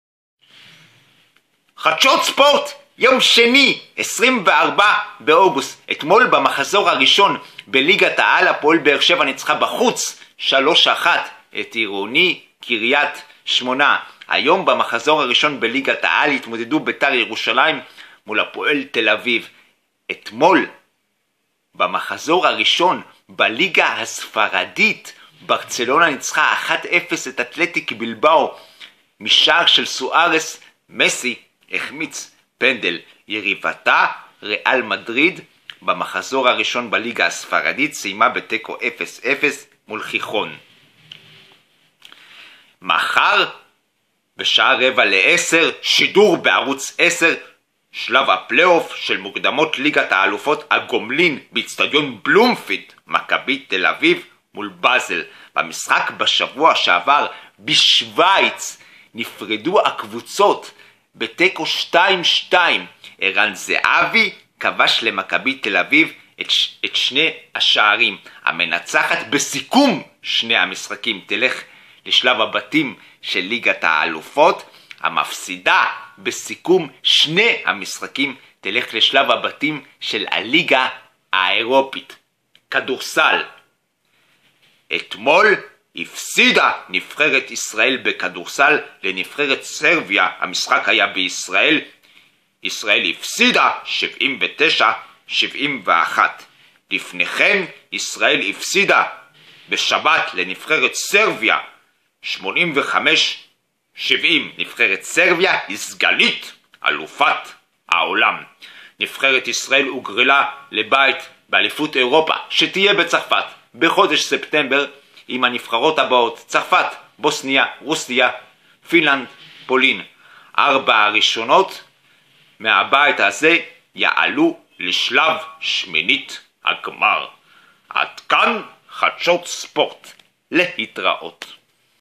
יום 135 חדשות ספורט שני 24.08.15 - מחזור 1 עירוני הפועל קרית שמונה 1 הפועל באר שבע 3
עדכון:במחזור הראשון  היום בית"ר ירושלים והפועל תל אביב נפרדו  בתיקו אפס בטדי.
מדליה כסף  לחנה  קנייזבה  מישראל  בקפיצה משולשת אליפות עולם